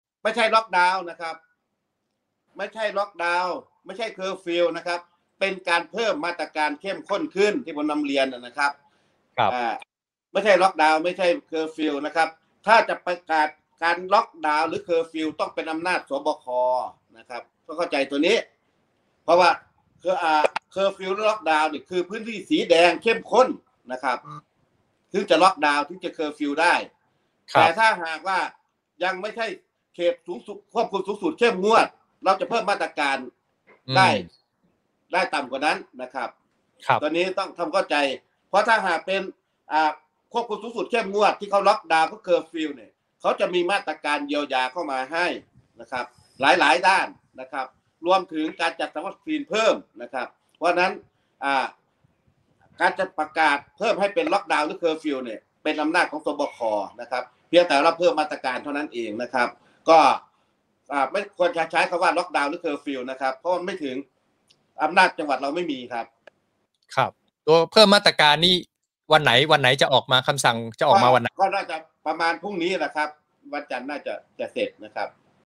ฟังชัดชัด จากปากรองผู้ว่าราชการจ.อุบลราชธานี กรณีล็อกดาวน์อ.เมือง,วารินฯ
รองผู้ว่าฯยัน ไม่มีล็อกดาวน์  ไม่มีเคอร์ฟิว มีเพียงมาตราการที่เข้มข้นขึ้น
คาดคำสั่งออกไม่เกินวันจันทร์ที่ 16 สิงหาคม 64

เกริกชัย ผ่องแผ้ว รองผู้ว่าราชการจ.อุบลราชธานี
ในการแถลงข่าว โควิดอุบลฯประจำวันที่ 15 สิงหาคม 2564